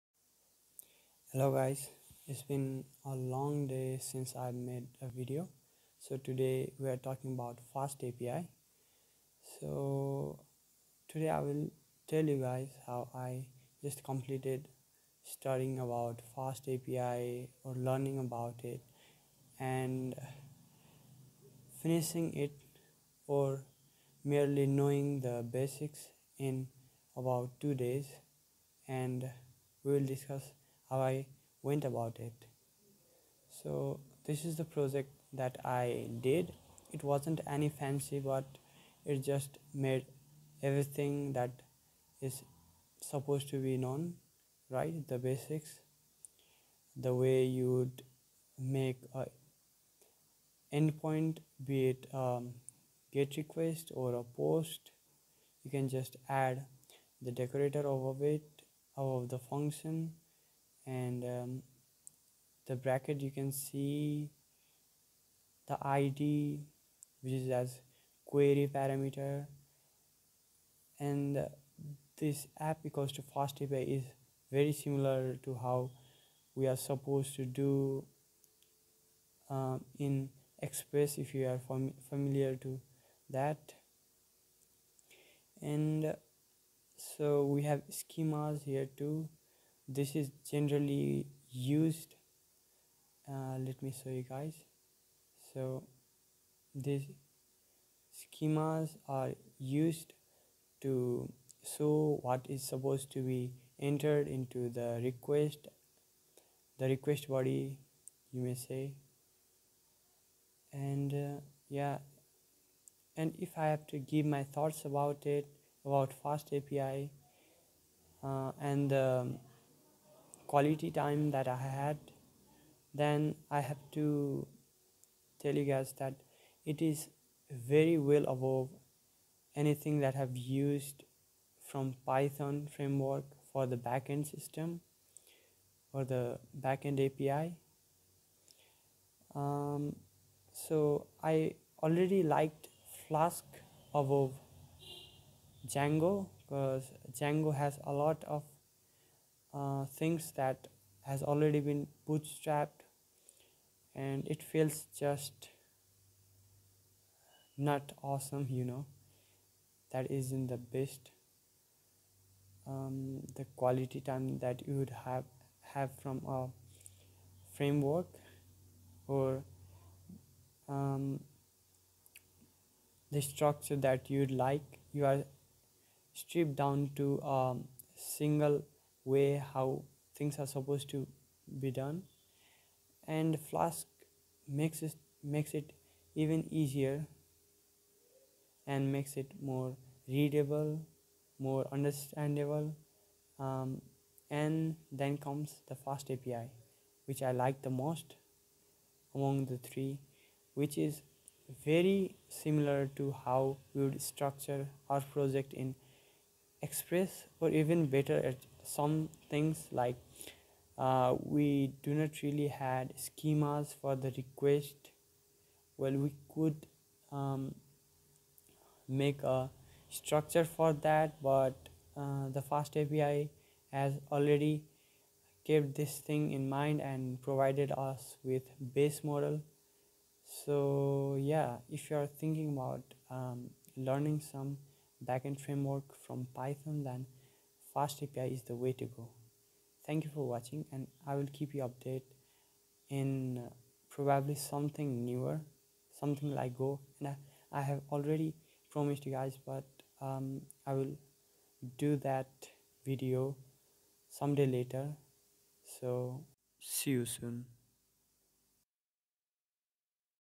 FastAPI learnt in 2 days || My thoughts about it
My experience of learning FastAPI in about 2 days, and actually liking it very much🎉🎉